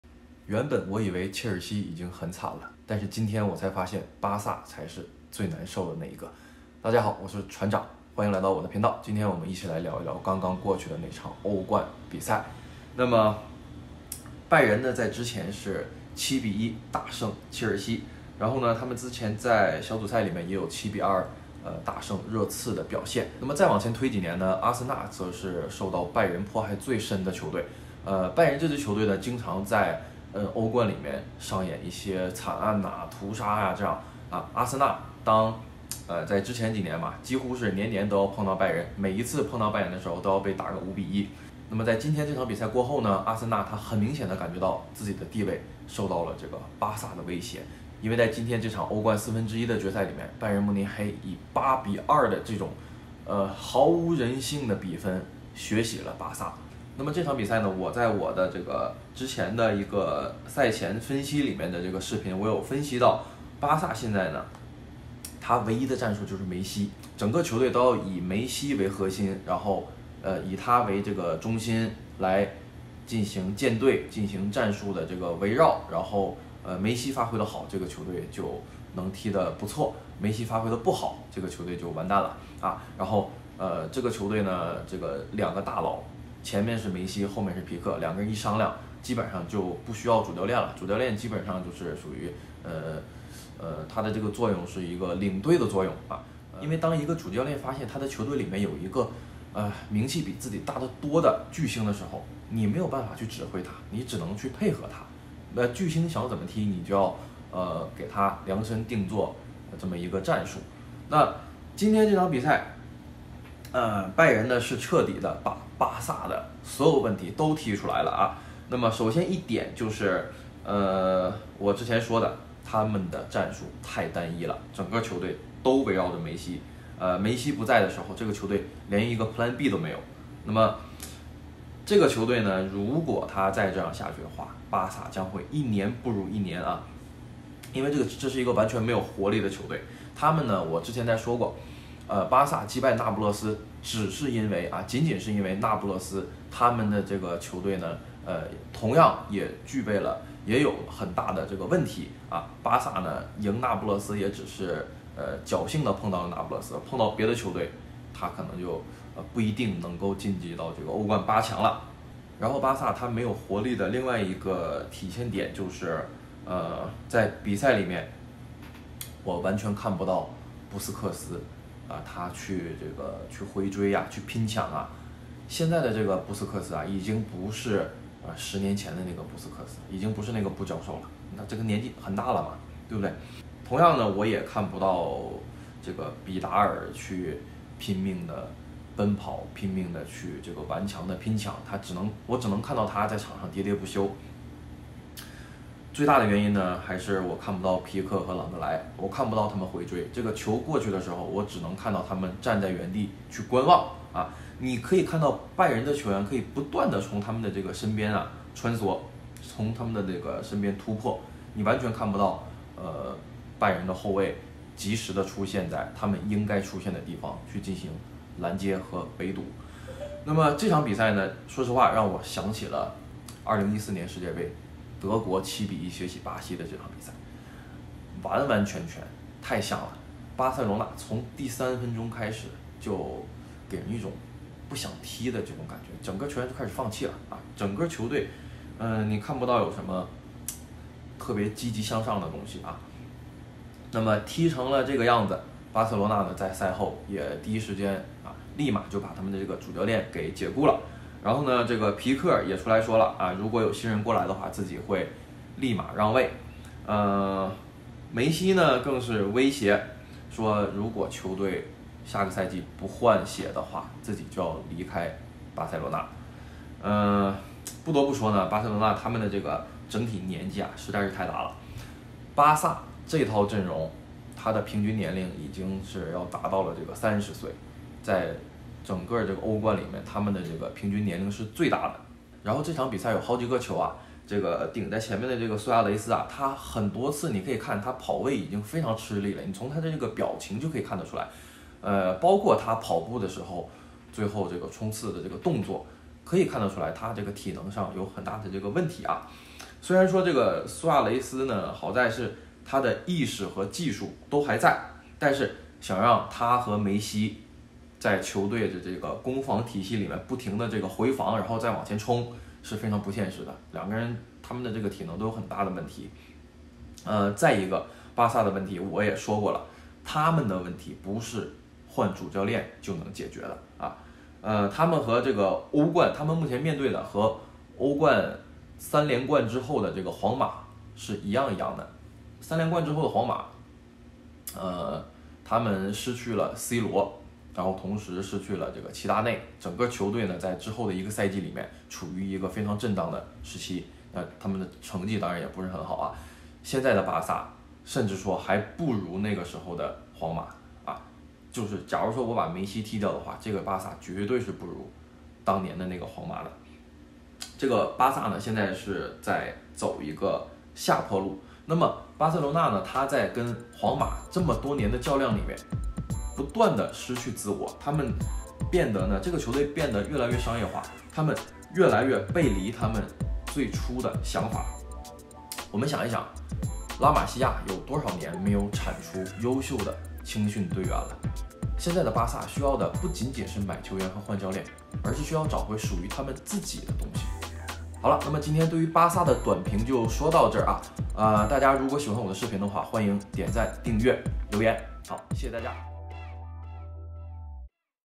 【欧冠】耻辱！巴萨2-8遭拜仁屠杀 梅西威胁要走 皮克愿意让位 背离了初衷 滑铁卢是必然
欧冠1/4 决赛 拜仁8-2 巴萨 创造最大惨案